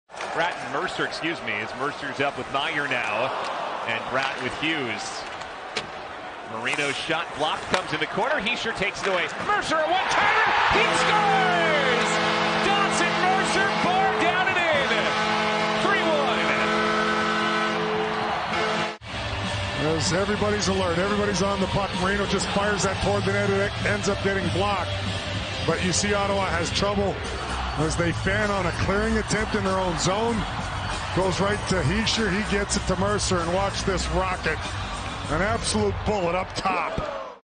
Mercer blasts one-timer
Dawson Mercer one-times Nico Hischier's feed into the net for a goal that makes it 3-1 in the 2nd period